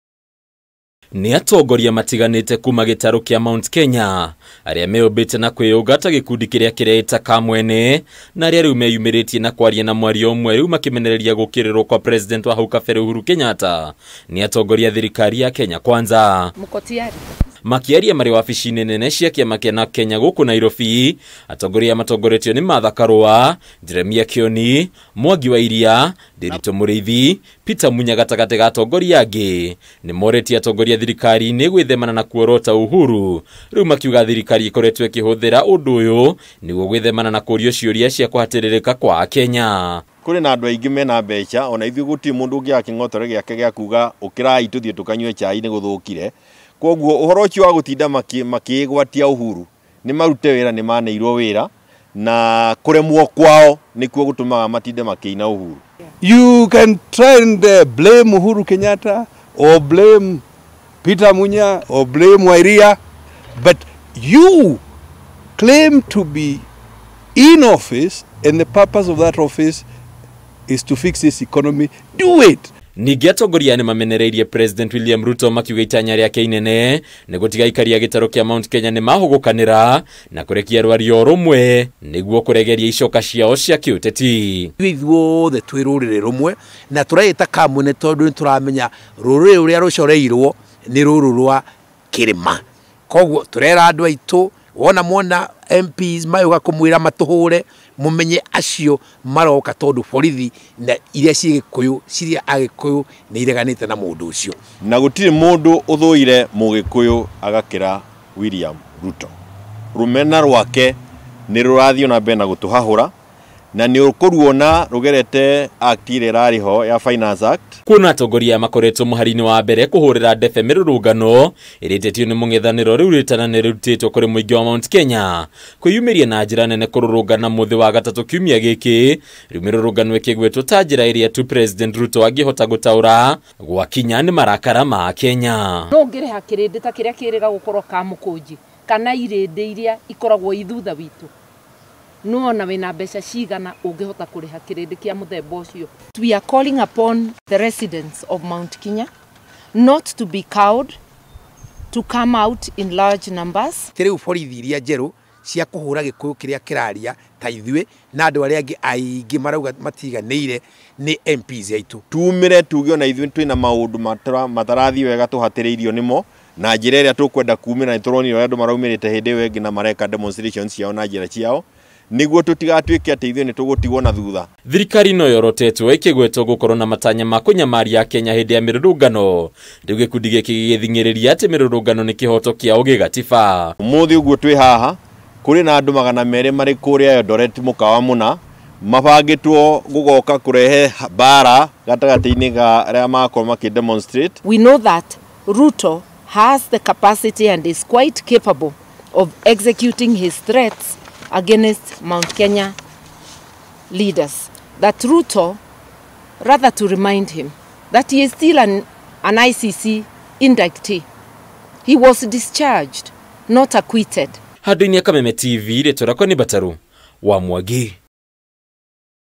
Tiganai na Uhuru na murute wira wanyu
ATONGORIA MATIGANITE KUUMA GITARU KIA MT. KENYA RIU NIMOKIRIIRE THIRIKARI YA KENYA KWANZA NA KUMIURIA ITIGE KUGIRITANIA MATHINA MARIA MAMIGUMIIRE NA PRESIDENT WA HAU KABERE UHURU KENYATTA, MAKIUGA UHURU NDARI HANDU ARAHUTANIA NA MATHINA MA THIRIKARI.
NINGI ATONGORIA AYA MAUGITE MAKORETWO MAGIIKARA NA KWIHUGA KUNENE MAGIITIGIRA KWAGAGIO KANA KWONIO NGANGA MBUTE NI THIRIKARI YA PRESIDENT WILLIAM RUTO RIU MAKIUGA THIRIKARI IRI NA ITANYA RIA KUMAGERA NGERO KUONA NIMARARIRIRIA MUKENYA URIA UTHIITE NAMBERE NA KUHATIRIRIKA.